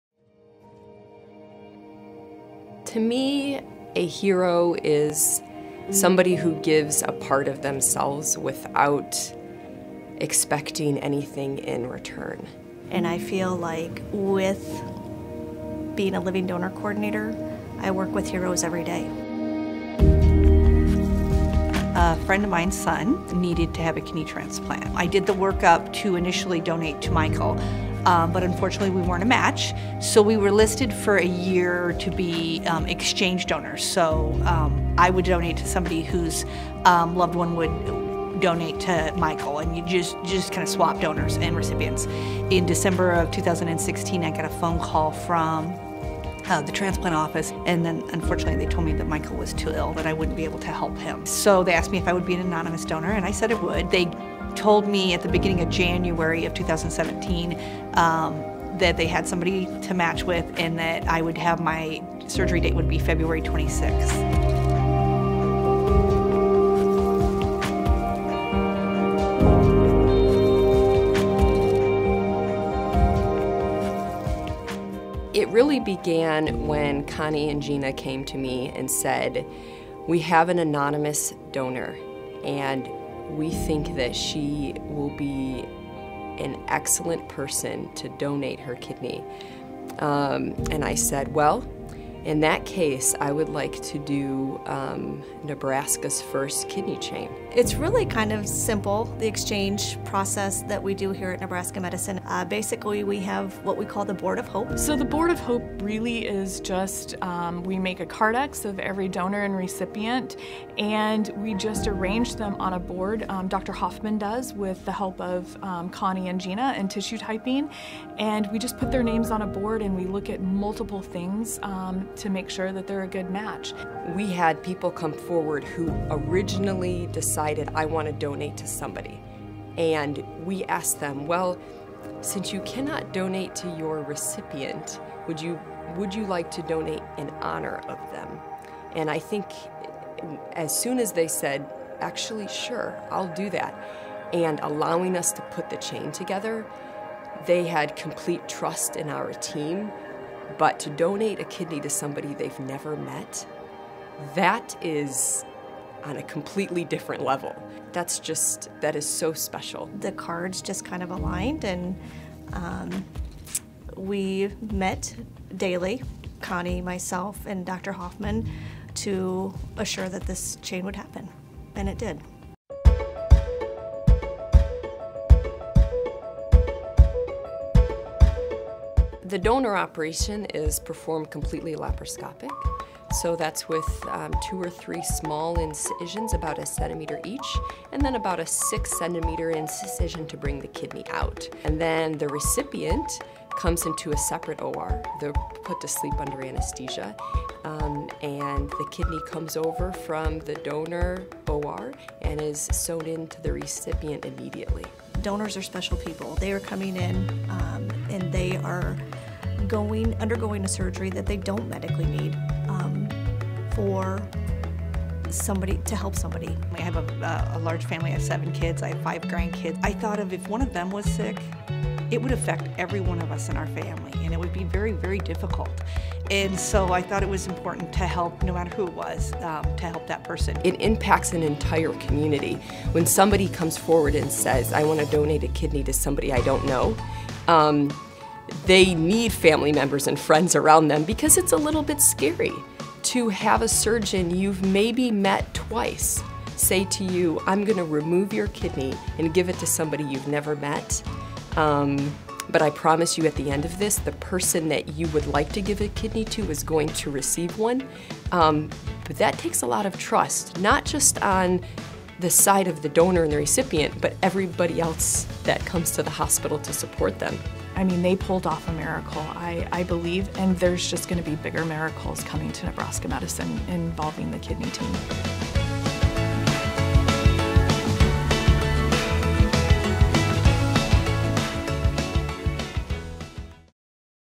Heroes in the Heartland: Gift of Life Heroes, Nebraska Medicine Kidney Transplant Team and Donors
Nebraska Medicine organized the state’s largest living donor transplant chain. Nine individual donors gave the greatest gift by donating a kidney to a stranger so that their loved one might receive a kidney. This amazing transplant chain took place over five consecutive days in the spring of 2017.